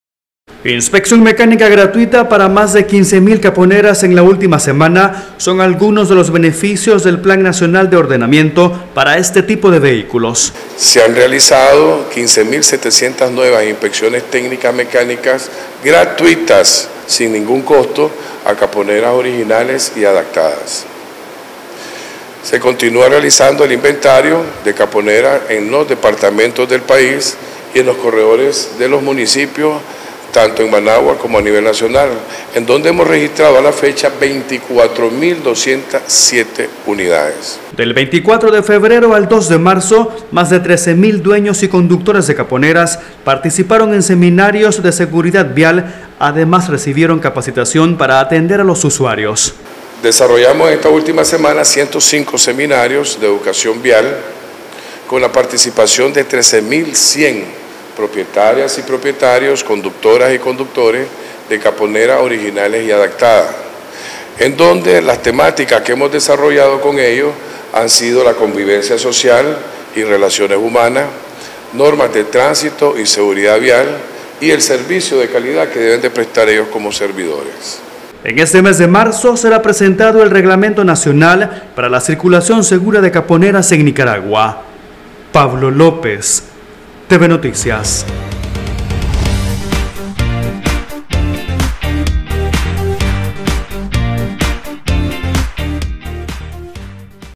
Más de 24 mil motos incluidas en inventario de caponeras en Nicaragua
Autoridades de la Policía Nacional presentaron los avances del Plan de Circulación Segura de conductores y pasajeros de caponeras en el período del 24 de febrero al 2 de marzo.